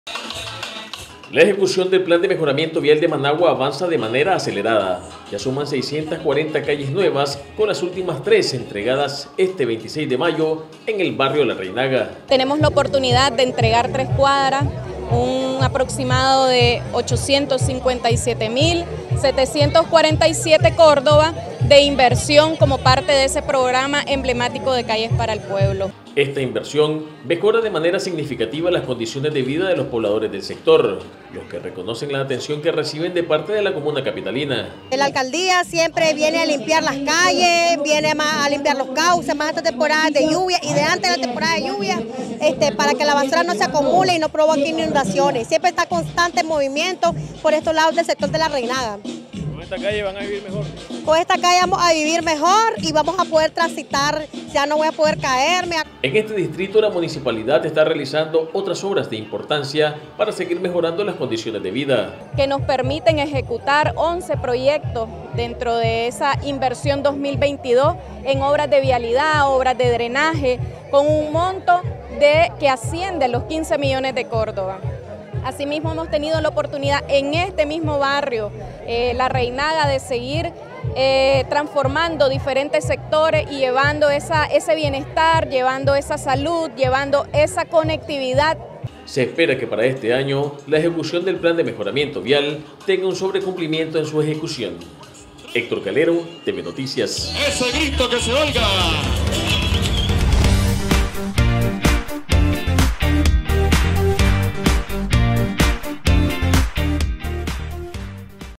Entregan 300 metros de calle en el barrio La Reynaga
Más calles son entregadas a las familias en la capital, esta vez los beneficiados fueron habitantes del barrio La Reynaga